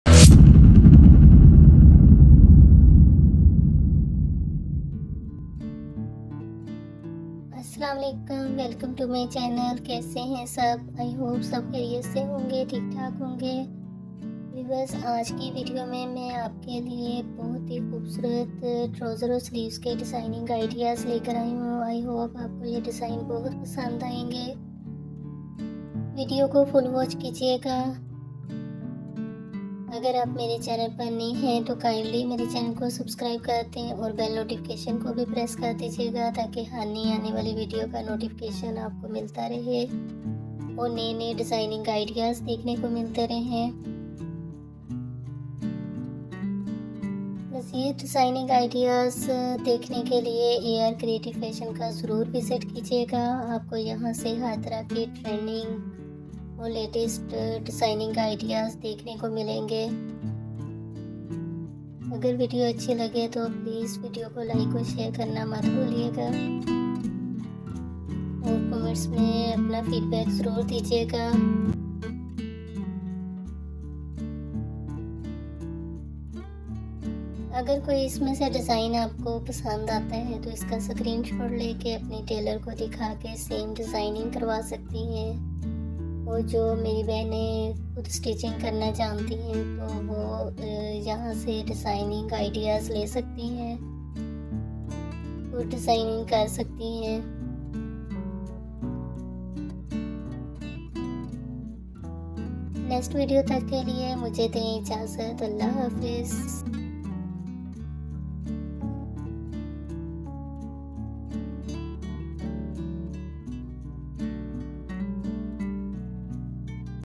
Winter Trousers and Saleaves Designing ideas|| Latest Fashion||ARCreativeFashion
Asslam.o.Alikum!
In this video I have shared very beautiful and Trendy Winter Trousers and Saleaves Designing ideas|| Latest Fashion||ARCreativeFashion
Shulwar k khoobsorat designing ideas
Bazi k khoobsorat designs
winter dresses p khoobsorat designing